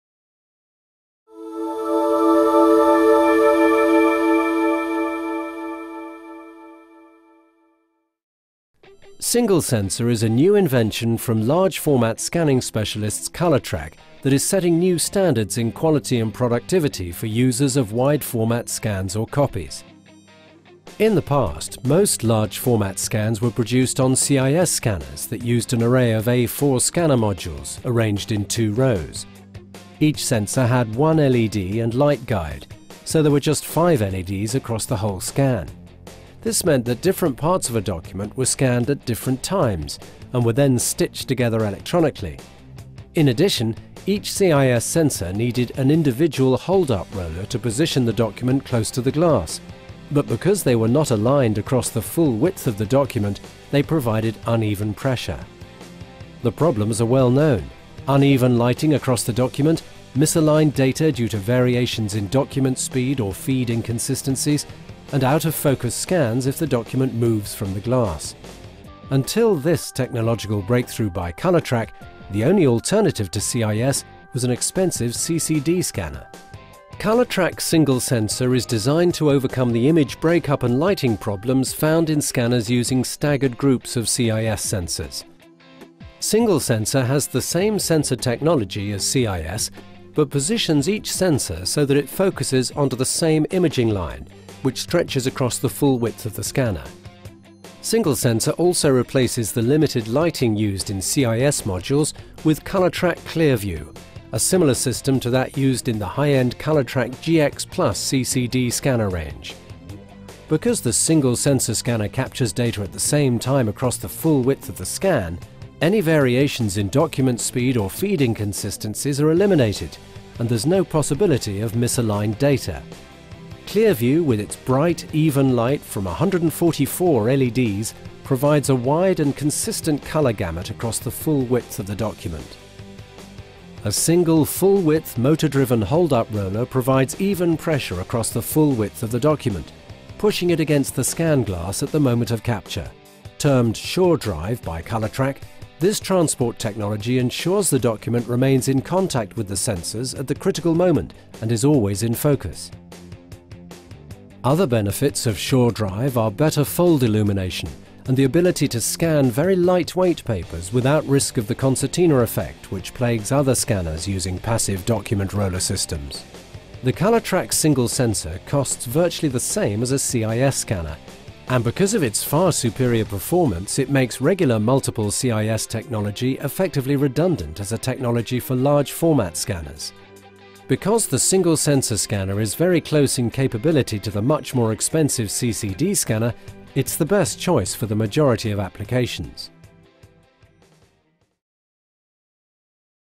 Colortrac SingleSensor
SingleSensor uses very similar basic technology to contact image sensor (CIS) scanners but instead of having separate rows of self-contained A4 sensors, Colortrac have completely re-invented the concept and produced a one-piece, rigid, single sensor strip with its own enhanced LED lighting system behind a single replaceable glass. This unique development has improved the quality obtainable from large format scanners to levels never seen previously.